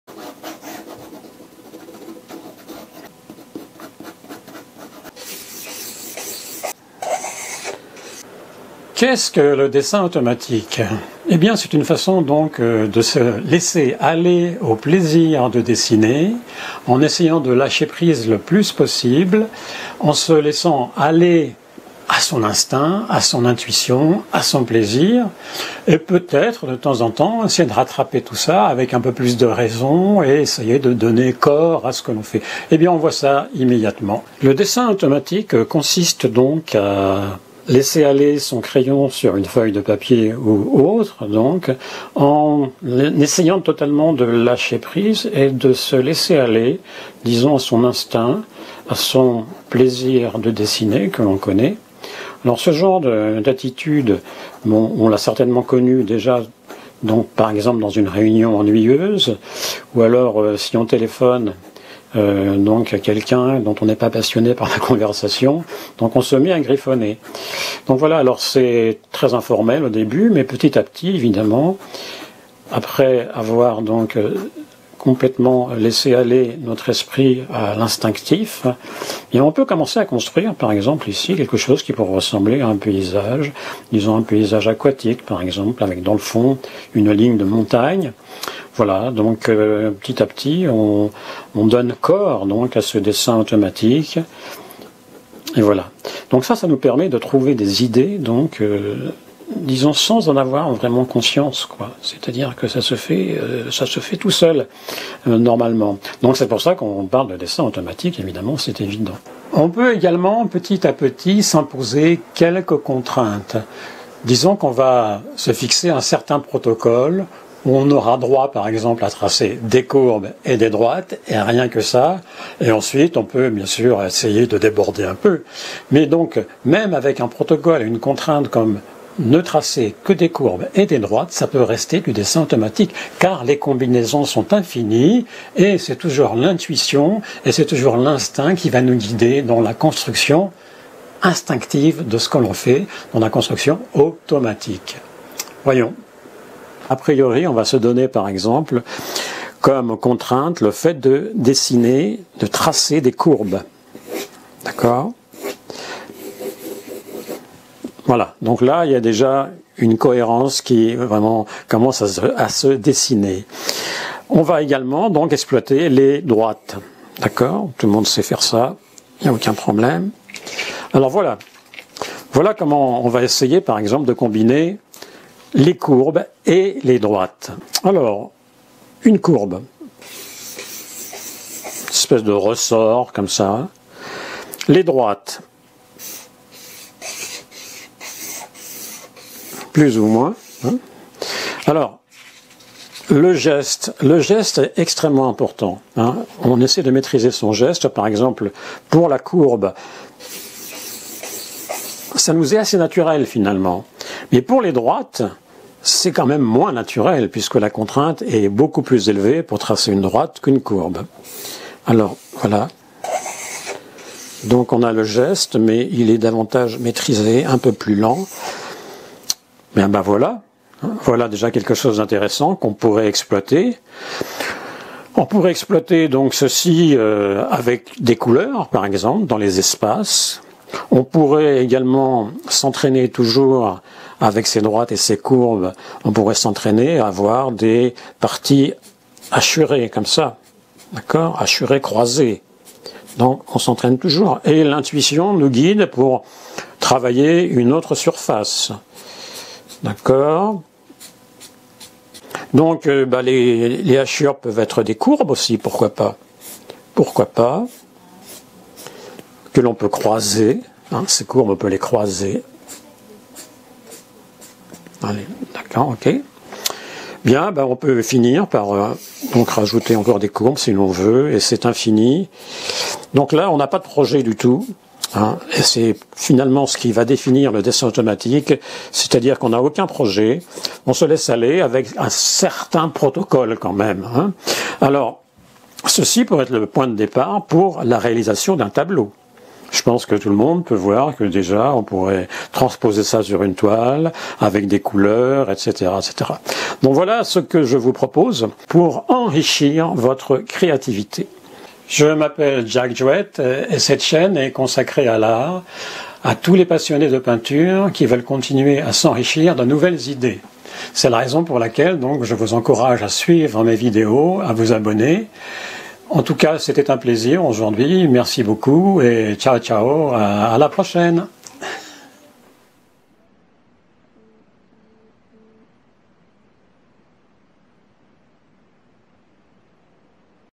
LES BASES DU DESSIN AVEC LE DESSIN AUTOMATIQUE. JACK JOUET COURS DE PEINTURE
CETTE VIDEO MONTRE ET EXPLIQUE LA TECHNIQUE DU DESSIN AUTOMATIQUE COMME BASE DE L'APPRENTISSAGE DU DESSIN.

1.)   DESSINER EN SUIVANT SON INSTINCT, SON INTUITION SANS AUCUNE CONTRAINTE, EN LACHANT PRISE, EN S'ADONNANT AU PUR PLAISIR DU DESSIN.

2.)   DESSINER EN S'IMPOSANT UNE CONTRAITE, UN PROTOCOLE. ICI ON S'IMPOSE DE NE DESSINER QUE DES COURBES ET DES DROITES QUI S'ENTRECROISENT.

LE BUT DU  DESSIN AUTOMATIQUE EST DE FOURNIR DES BASES A L'APPRENTISSAGE DU DESSIN.